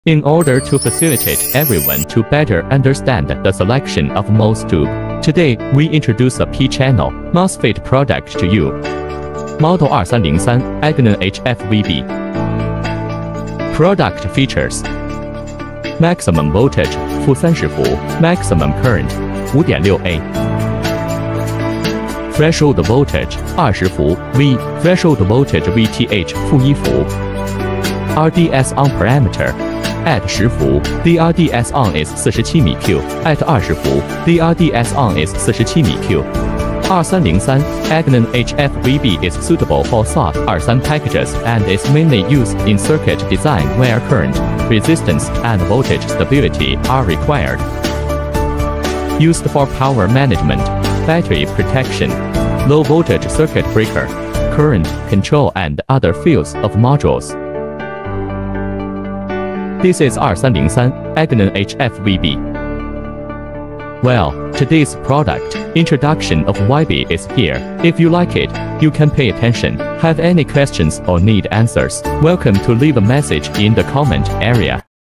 2303AGN-HF-VB is a SOT23 package P-Channel field effect MOS tube
"**Product model:** 2303AGN-HF-VB

**Silkscreen:** VB2355

**Brand:** VBsemi

**Parameters:**
- Package type: SOT23
- Channel type: P—Channel
- Maximum withstand voltage: -30V
- Maximum current: -5.6A
- Turn-on resistance: 47mΩ @ VGS=10V, VGS=20V
- Threshold voltage: -1V

**Package:** SOT23

- **Channel Type:** The product adopts P—Channel channel, which is suitable for specific application scenarios.
- **Maximum withstand voltage:** The maximum withstand voltage of the product is -30V, which enables it to work stably under different power supply environments.
- **Maximum Current:** The product supports a maximum current of -5.6A, suitable for applications with higher current requirements.
- **On-Resistance:** At VGS=10V and VGS=20V, the product's on-resistance is 47mΩ, showing a low on-resistance, which helps improve efficiency.
- **Threshold Voltage:** The product's threshold voltage is -1V, enabling it to operate normally under low voltage conditions.

**Application Introduction:**
2303AGN-HF-VB is suitable for SOT23 packaging and is mainly used in circuit designs with high requirements for current, resistance and voltage stability. Common application areas and modules include:
- **Power Management Module:** Due to its P-Channel channel and low on-resistance, it can be used in power management modules to provide effective current control.
- **Battery Protection:** In circuits where battery protection is required, this product can be used to ensure reliable battery control under different working conditions.
- **Low voltage circuit breaker:** Due to the threshold voltage of -1V, it is suitable for the design of low voltage circuit breakers to ensure reliable triggering under low voltage conditions.
- **Current control module:** Due to the maximum current of -5.6A, it can be used in circuit designs that require higher current control, such as motor control and drive modules.

These application areas show that this product has a wide range of application prospects in circuit designs that require P-Channel channels, low resistance and high current."